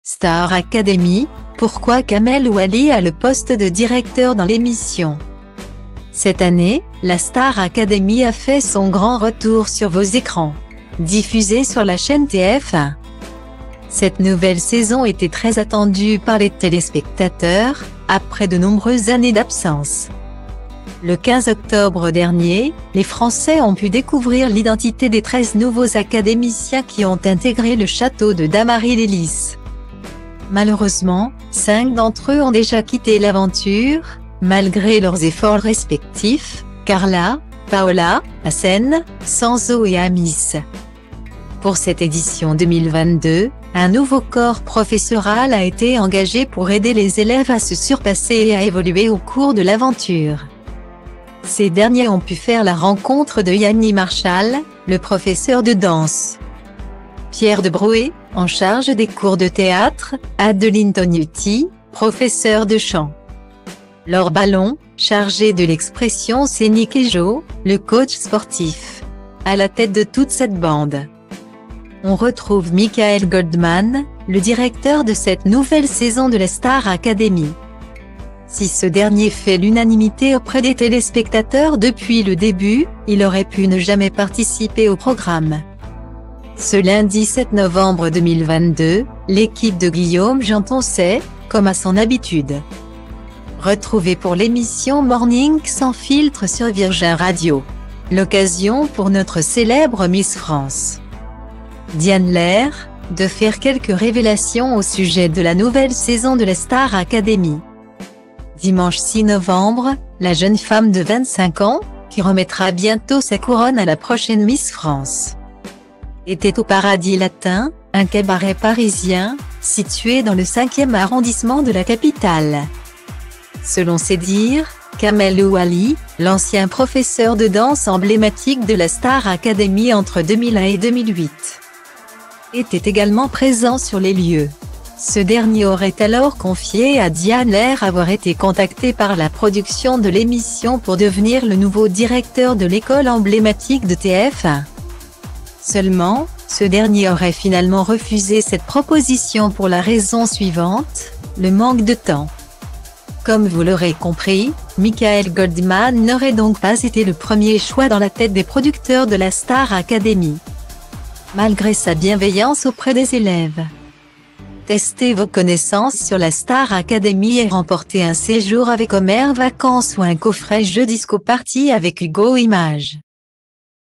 Star Academy : pourquoi Kamel Ouali a le poste de directeur dans l'émission
Cette année, la Star Academy a fait son grand retour sur vos écrans. Diffusée sur la chaîne TF1,cette nouvelle saison était très attendue par les téléspectateurs, après de nombreuses années d'absence.Le 15 octobre dernier, les Français ont pu découvrir l'identité des 13 nouveaux académiciens qui ont intégré le château de Dammarie-lès-Lys.